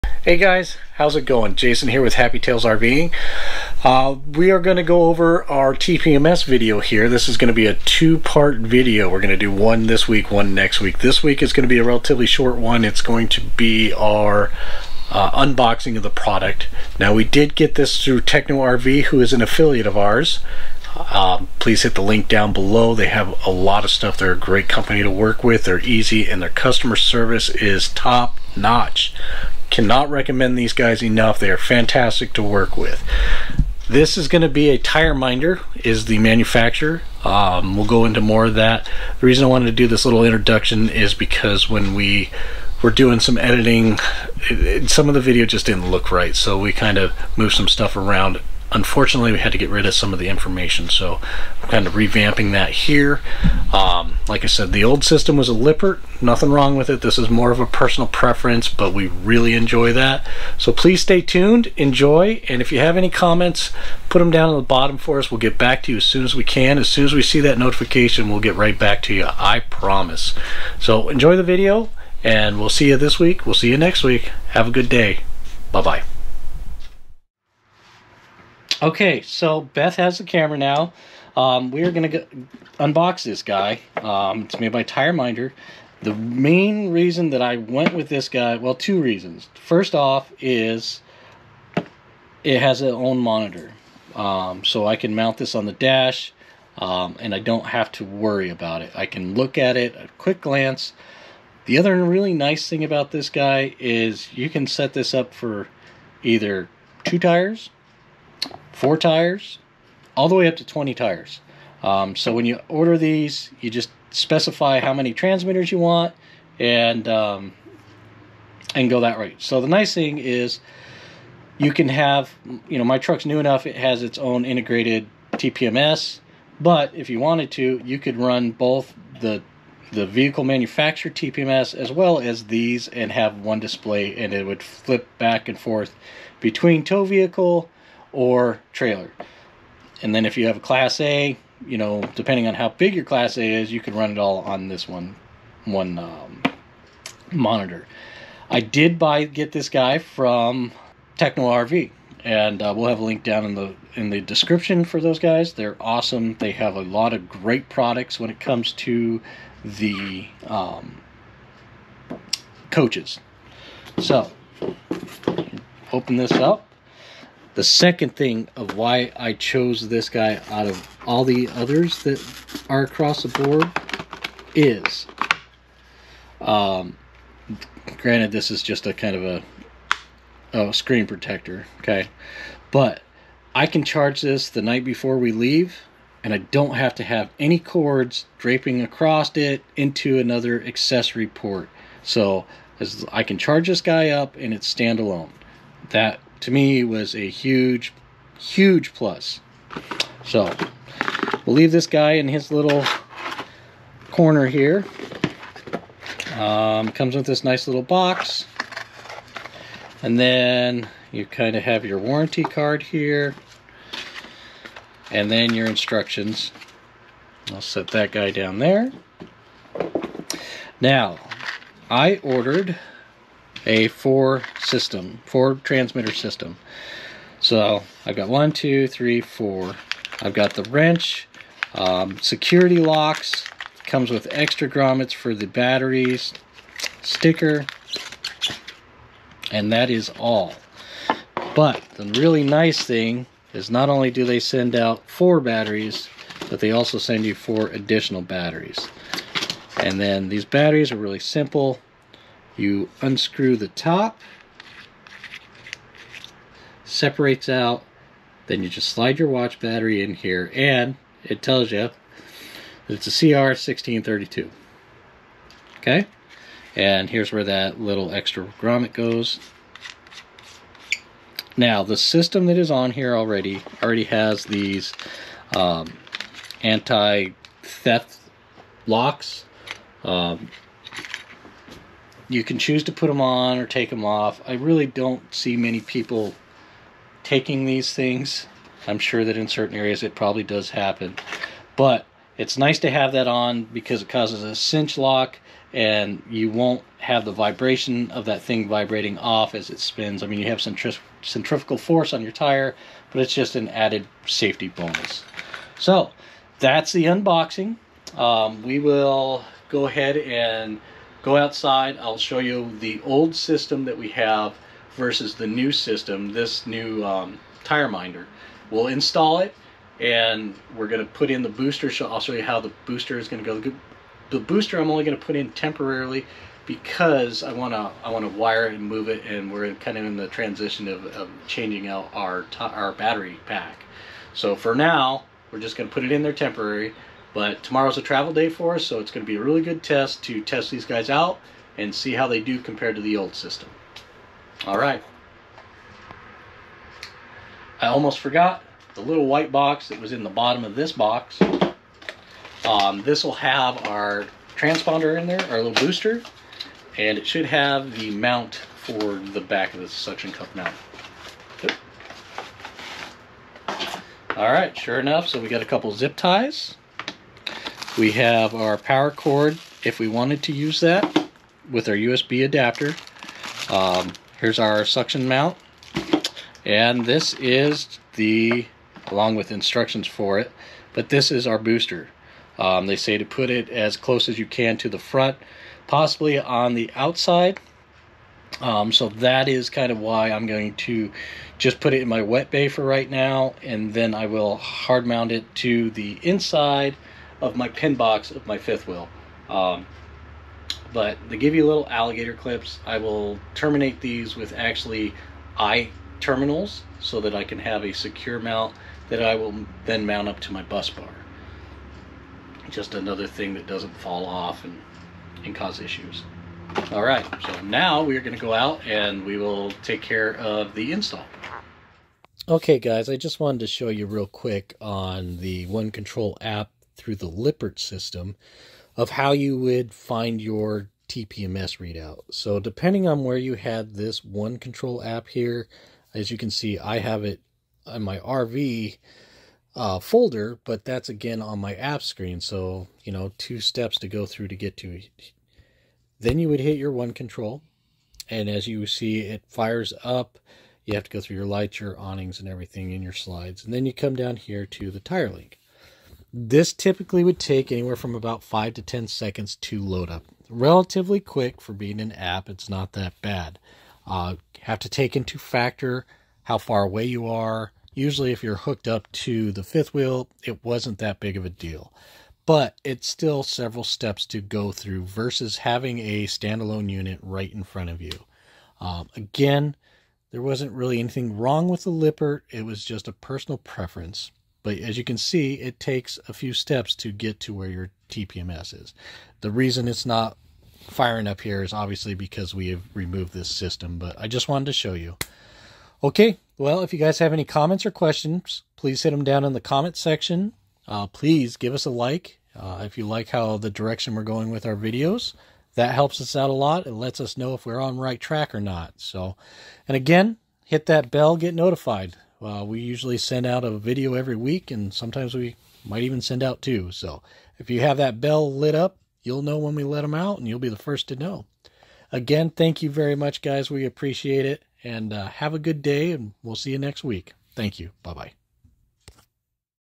Tire Minder TPMS Unboxing ( Why We Switched )
Hello Everyone.  Todays video we will be doing an unboxing of the Tire Minder TPMS.  We will explain why we switched from our factory TPMS to the Tire Minder, and why we like it so much.

Links
TPMS -

P.S.  Check out some other awesome products from Techno RV.!!!